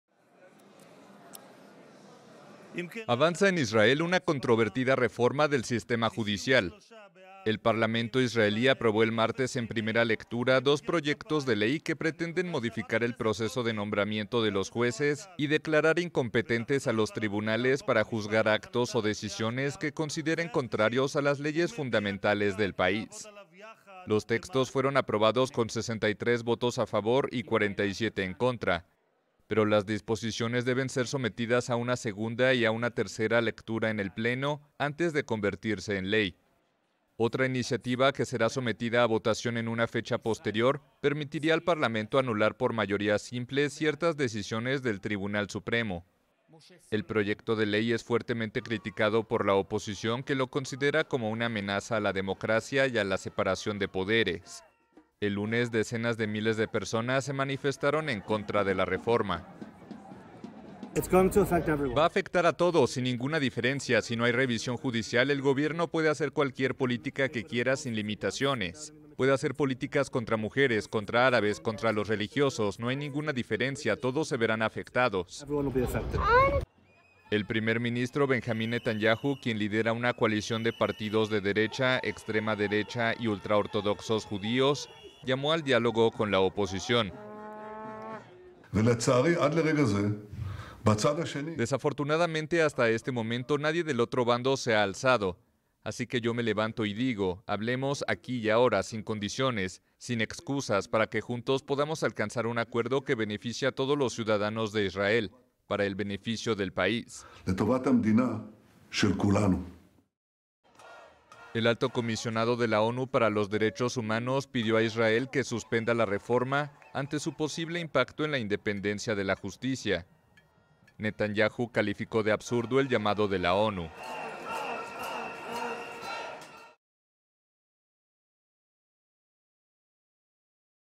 Polémica reforma judicial en Israel pasa una primera etapa | AFP
El Parlamento israelí aprobó el martes en primera lectura dos proyectos de ley de una controvertida reforma del sistema judicial considerada por sus opositores como una amenaza a la democracia.